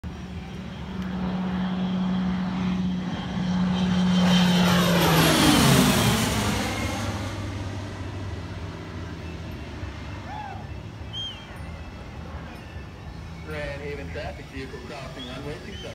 King Air sunset flyby
Flyby after a great Saturday of jumping!